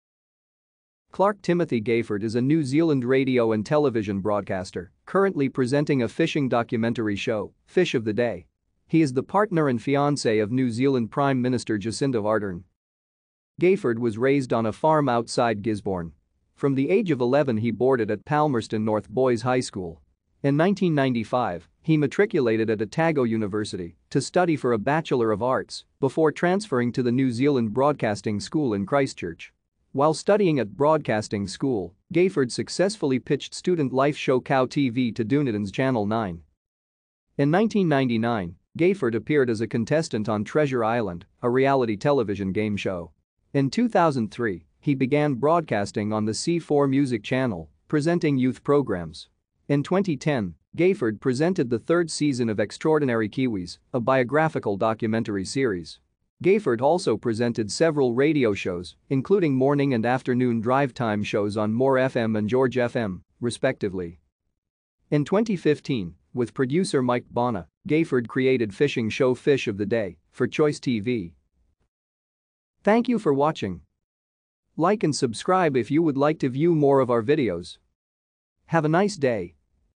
Clarke Gayford
Clarke Timothy Gayford  is a New Zealand radio and  broadcaster, currently presenting a fishing documentary , Fish of the Day. He is the partner and fiancé of New Zealand Prime Minister Jacinda Ardern.